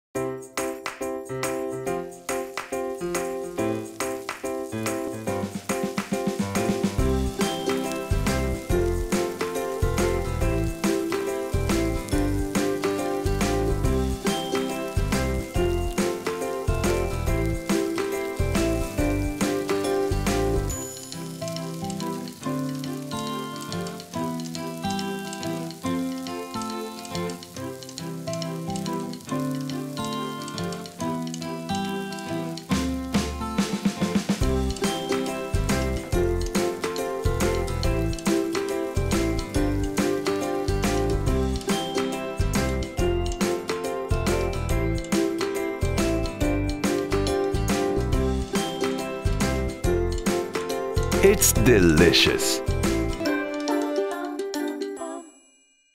Fried Eggs with Cheese Sauce
Heat the vegetable oil in a skillet and fry the eggs. Season them with salt.
Fry the bacon slices in a skillet, without using vegetable oil.
Put the heavy cream in a saucepan. Add the Cheddar cheese and the mozzarella.
Cook them together until the cheese melts.
Crumble 3 slices of fried bacon in small pieces and add them in the cheese sauce. If you want the sauce to be thicker, add some more Cheddar.
Place the fried eggs on a plate and top them with cheese sauce.
Crumble the remaining fried bacon and sprinkle it over the eggs.
Before you serve the eggs, add some chopped dill.